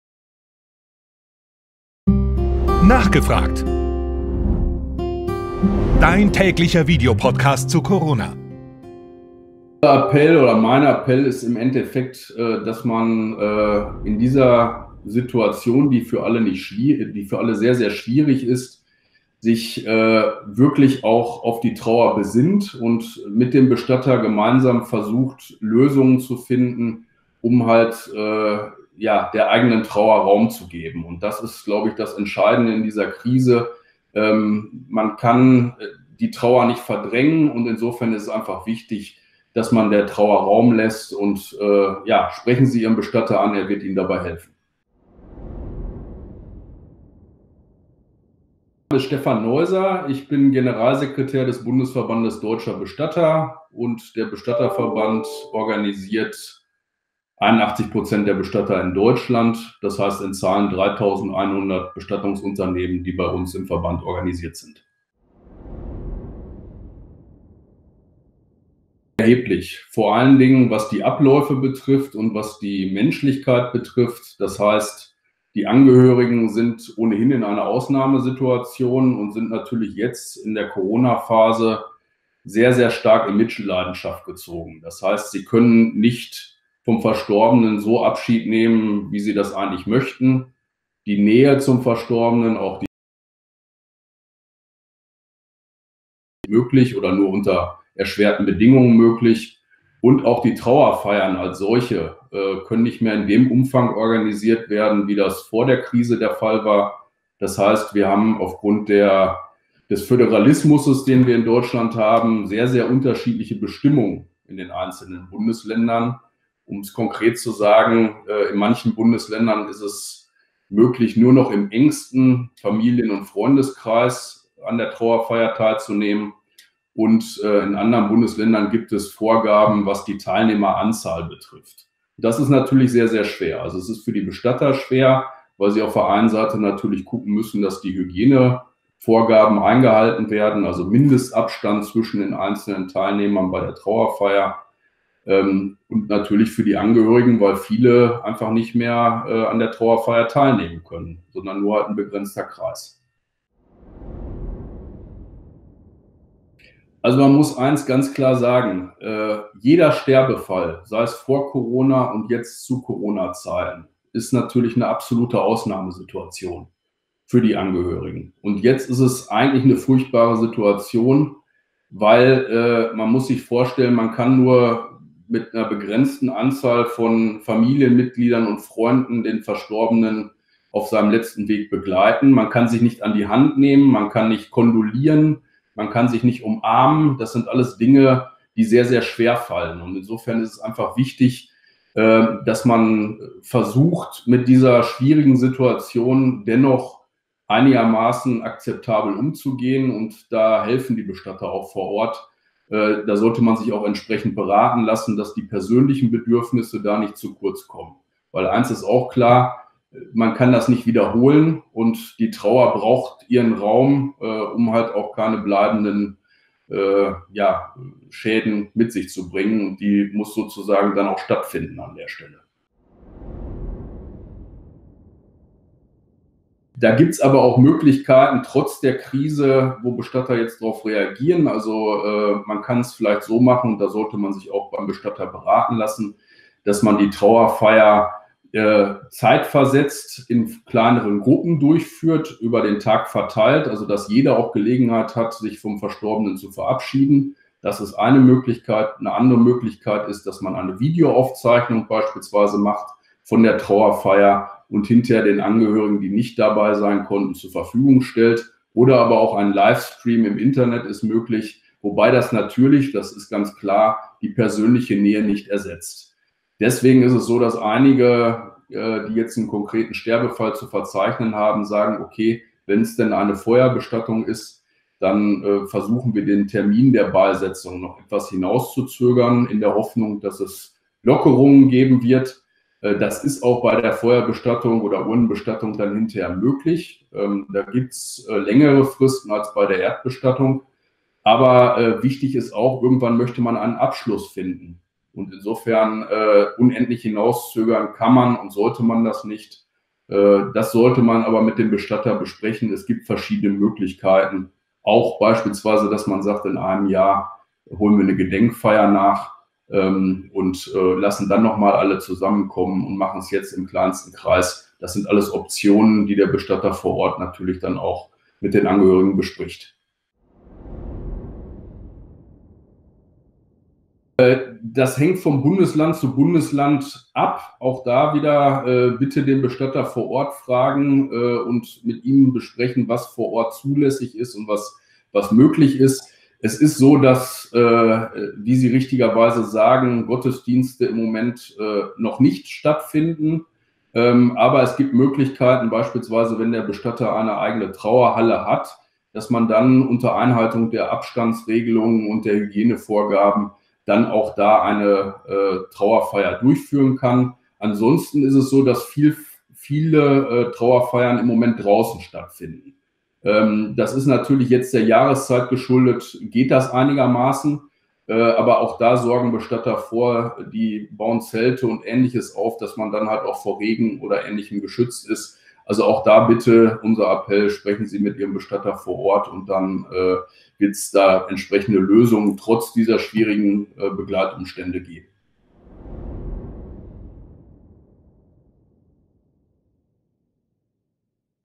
Nachgefragt! Interview mit Stephan Neuser, Bund Deutscher Bestatter e.V.
Abschied nehmen ist ein besonderer Prozess. In Zeiten von Corona hat sich dabei allerdings einiges verändert. Wie man trotzdem eine würdevolle Trauerfeier abhalten kann, erklärt uns Stephan Neuser vom Bund Deutscher Bestatter e.V.. Weitere Informationen zu Corona findet ihr